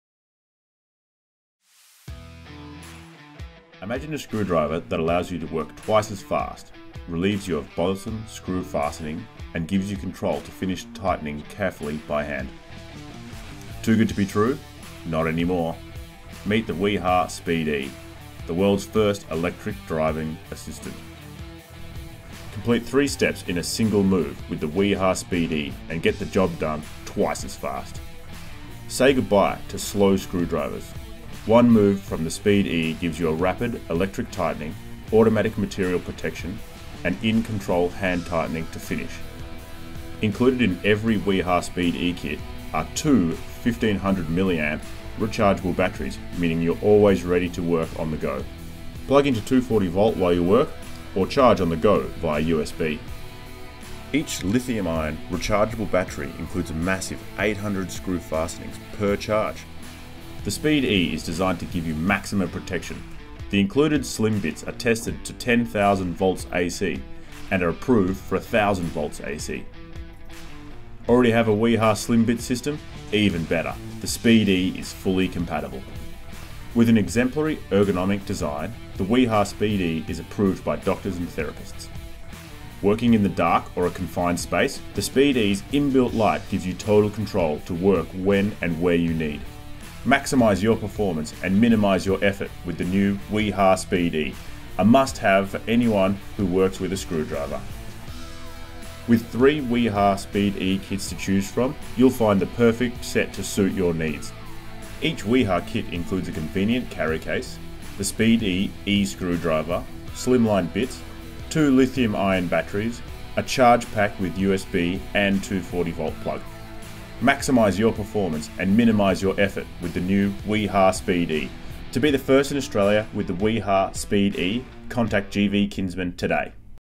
Wiha SpeedE Screwdriver Distributed by GVK - Features & Benefits
Launching in August 2018!
 "Drive with speed, fix with feeling"

As the first VDE-certified e-screwdriver from Wiha, speedE® promises to halve the time users take to complete their work in the future. An electric motor assists with fastening screws up to 0.4 Nm before disengaging to ensure that material is protected.